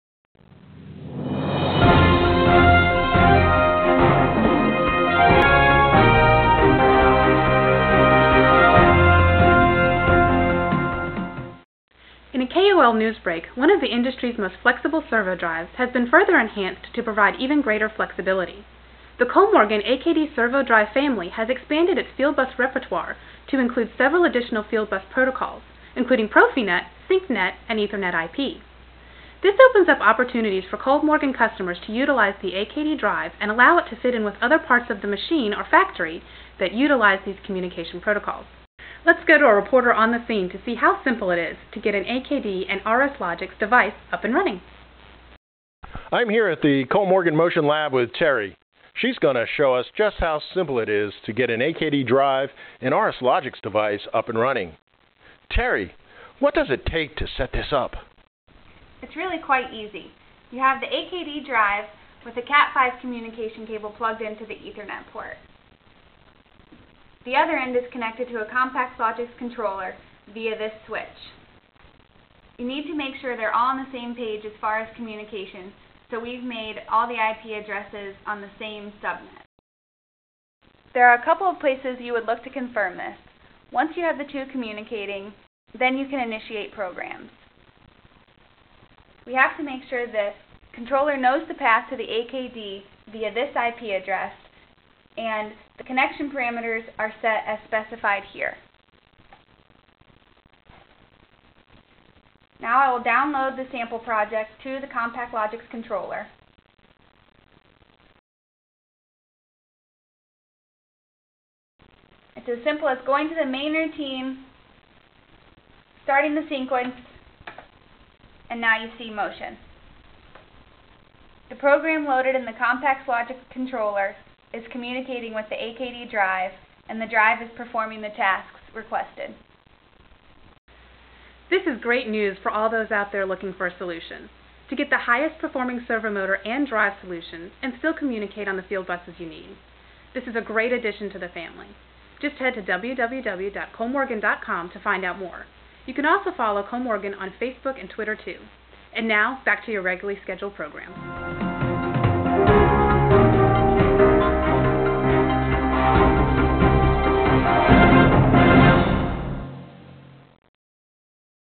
Kollmorgen AKD SPECIAL REPORT.mov
Kollmorgen has further enhanced one of the industry's most flexible servo drives. The AKD servo drive family expanded its repertoire to include several additional field test protocols, including: Profinet, SynqNet, and EtherNet/IP. Watch this video to see just how easy it is to get an AKD drive and RSLogix device up and running.